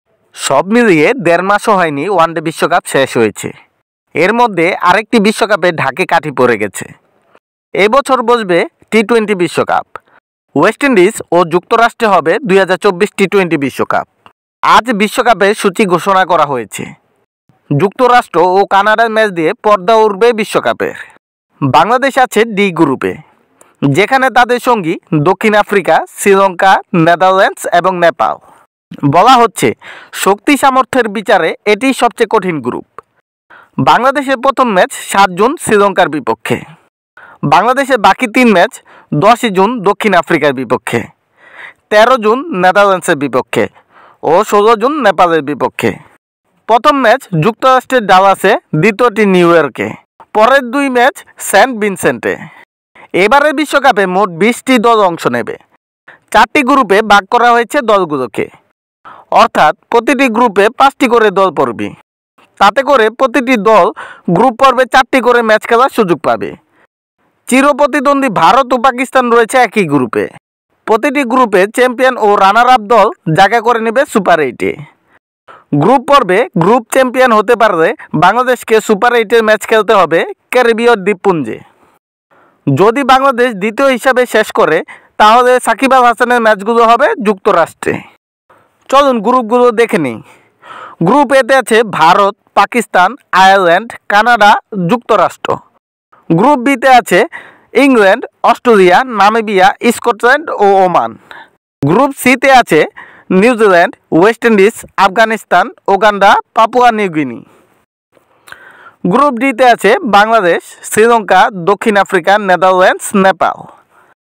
২০২৪ টি-টোয়েন্টি বিশ্বকাপের ড্র ; গ্রুপ-ডি তে শক্তিশালী প্রতিপক্ষের গ্রুপে বাংলাদেশ।
২০২৪ টি-টোয়েন্টি বিশ্বকাপের ড্র ; গ্রুপ-ডি তে শক্তিশালী প্রতিপক্ষের গ্রুপে বাংলাদেশ।